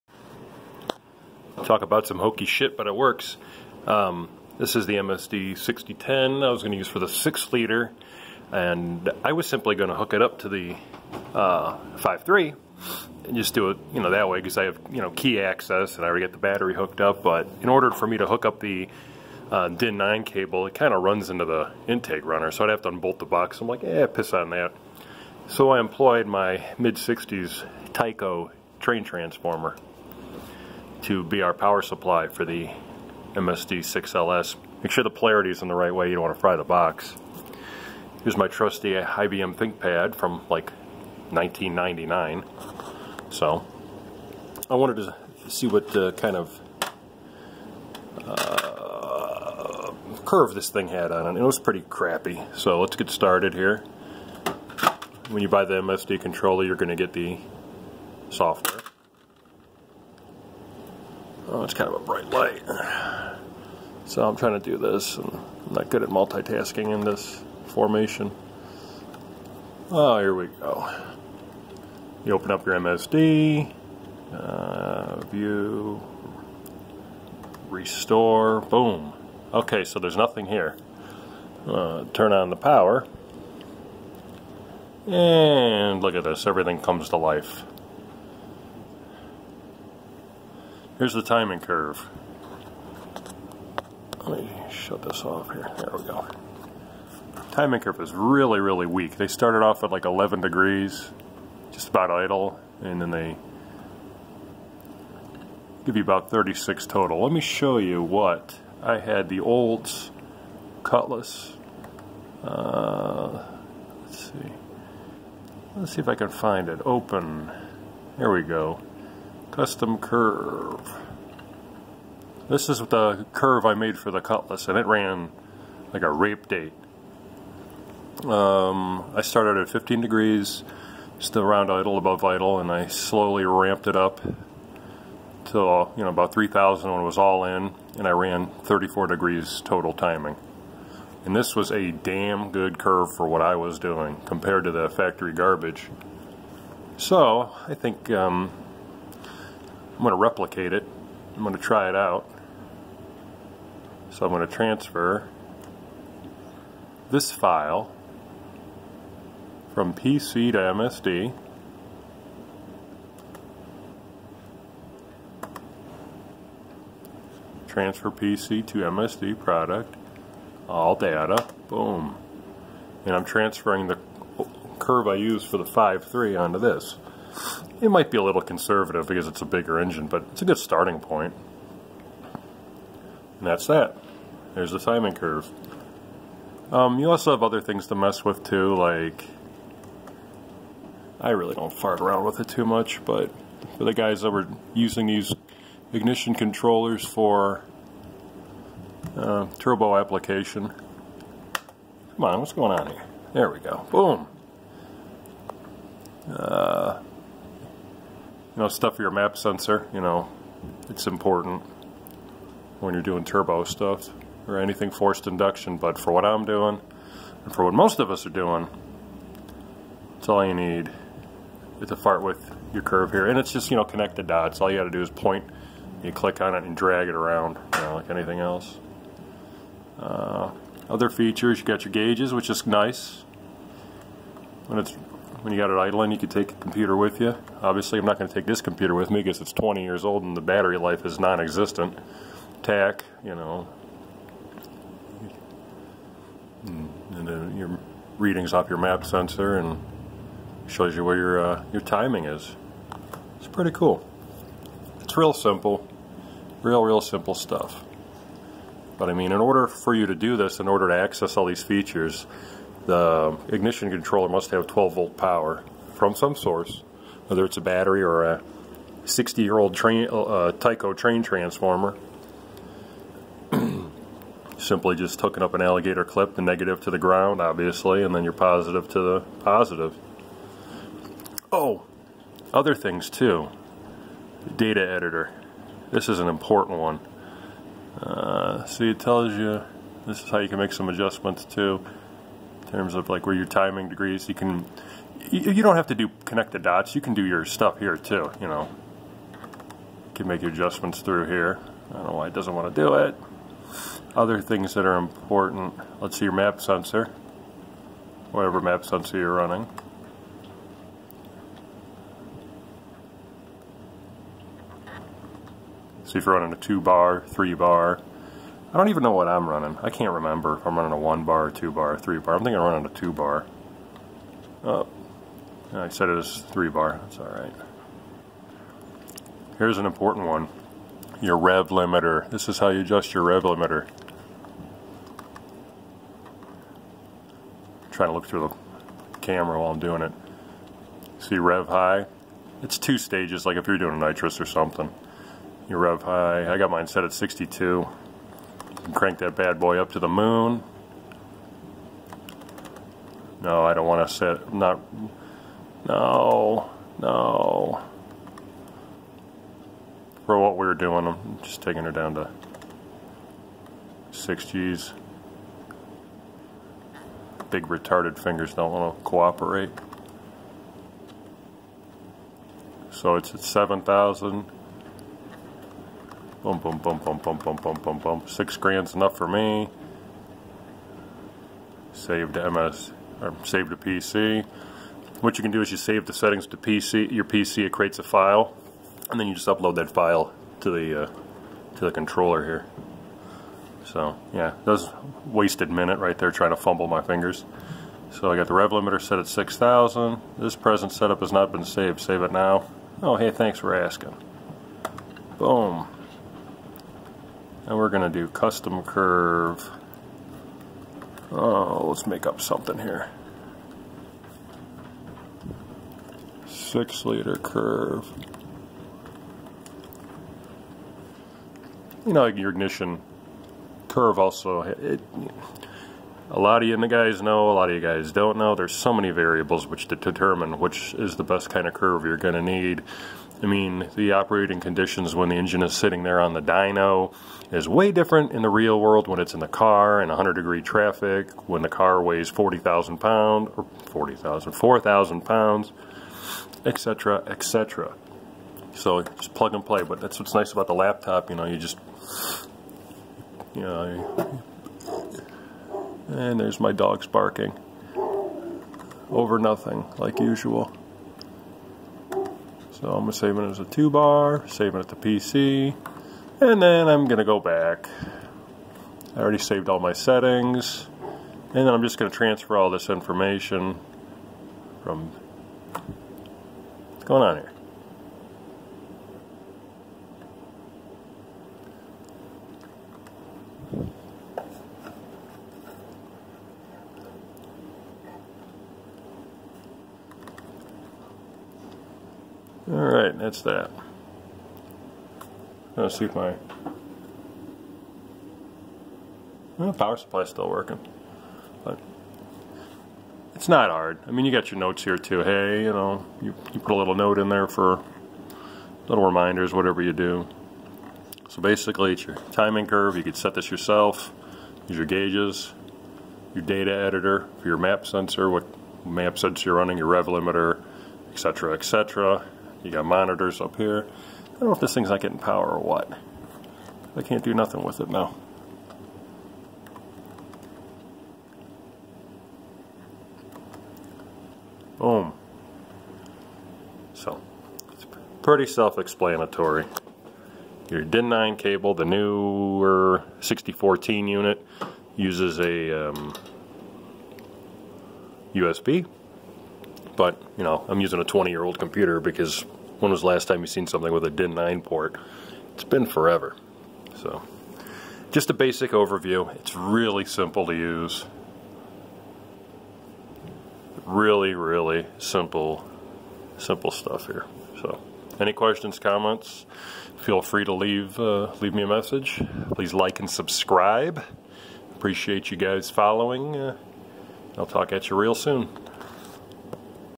MSD-6010 ignition controller basic overview! Tuning via PC. Rev limiter, custom curve etc.
This is for the MSD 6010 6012 and 6014.
I'm going over the basic over view in regards to using the program. Making a  custom curve is as simple as connect the dots. I start off around 15 degrees at idle ramping up to full advance by 3000 rpms. This works for me and what I'm doing. Doing this on the dyno is a no brainier but for a basic street performance tune I like it.

I cover making the appropriate  Map Sensor setting as well as setting your rev limiter

By using the PC and creating a custom curve you can now throw out those useless pills. They give way too much total timing. The engines don't need it because of their superior combustion Chambers.

Msd6010
MSD6012
Msd2014